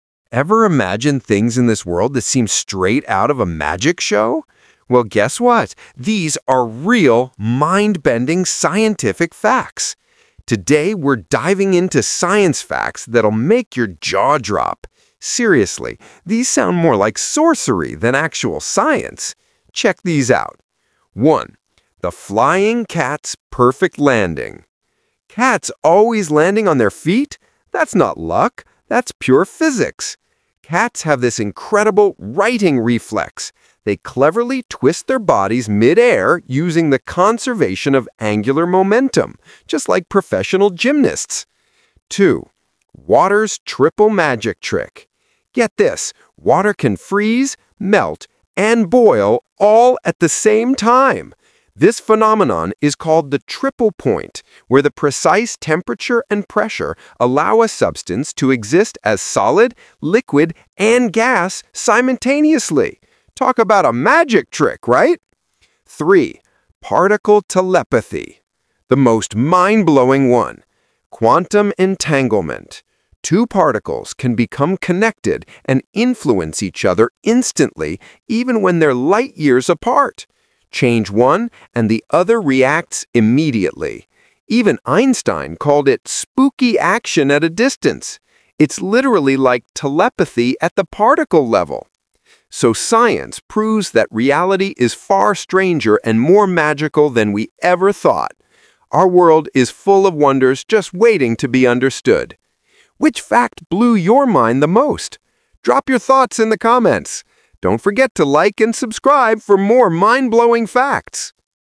Science Facts That Sound Like Magic!
Some scientific phenomena are so incredible, they seem to defy logic and belong in fantasy stories rather than our reality.

In this Short, we explore three mind-bending scientific facts that feel like pure magic:

- The Physics of Cat Landings: How cats use the "righting reflex" and angular momentum to always land on their feet.
- Water's Triple Point: The magical state where water exists as solid, liquid, and gas simultaneously.
- Quantum Entanglement: Einstein's "spooky action at a distance" where particles communicate instantly across vast distances.